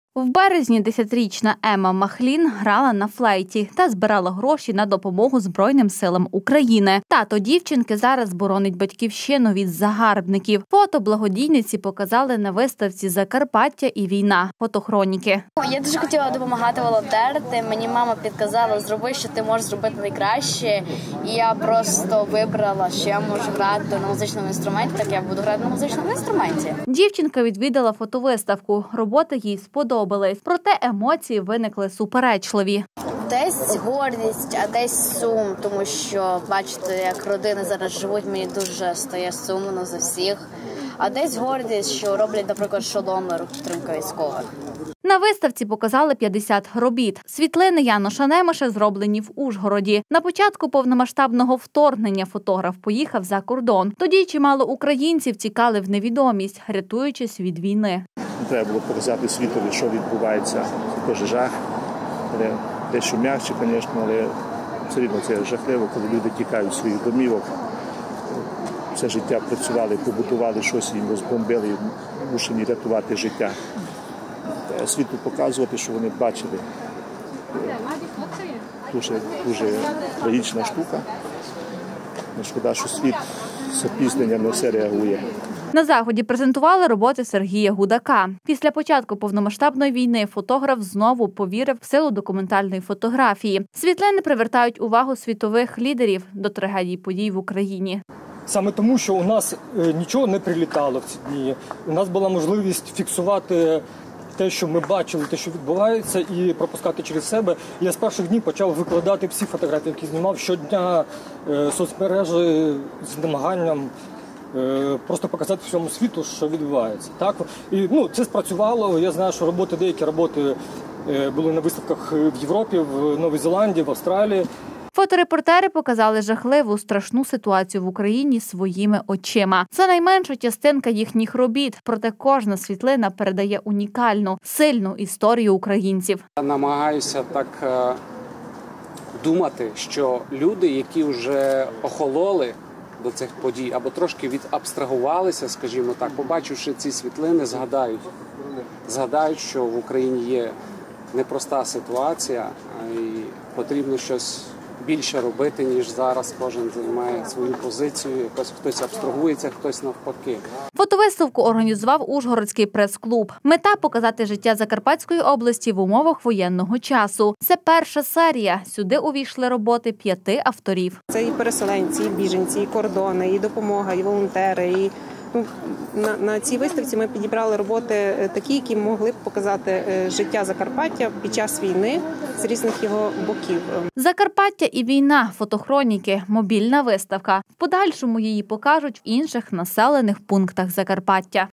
Закарпаття і війна. Фотохроніки
В Ужгороді відкрили виставку «Закарпаття і війна. Фотохроніки». Показали роботи п’яти авторів, які висвітлювали життя регіону під час війни. На світлинах кордони, переселенці, біженці, гуманітарна допомога та волонтери. Виставку організував Ужгородський прес-клуб. В подальшому, побачити Закарпаття під час війни очима фотокореспондентів можна буде і в інших населених пунктах області. В березні десятирічна Ема Махлін грала на флейті та збирала гроші на допомогу збройним силам України. Тато дівчинки зараз боронить Батьківщину від загарбників. Фото благодійниці показали на виставці «Закарпаття і війна. Фотохроніки.» Дівчинка відвідала фотовиставку. Роботи їй сподобались, проте, емоції виникли суперечливі. На виставці показали 50 робіт. Світлини Яноша Немеша зроблені в Ужгороді. На початку повномасштабного вторгнення фотограф поїхав на кордон. Тоді чимало українців тікали в невідомість, рятуючись від війни. На заході презентували роботи Сергій Гудак. Після початку повномасштабної війни фотограф знову повірив в силу документальної фотографії. Світлини привертають світову увагу до трагічних подій в Україні. Фоторепортери показали жахливу, страшну ситуацію в Україні своїми очима. Це найменша частина їхніх робіт, проте, кожна світлина передає унікальну, сильну історію українців. Фотовиставку організував Ужгородський прес-клуб. Мета – показати життя Закарпатської області в умовах воєнного часу. Це перша серія – сюди увійшли роботи п’яти авторів. «Закарпаття і війна. Фотохроніки.» -  мобільна виставка. В подальшому її покажуть в інших населених пунктах Закарпаття.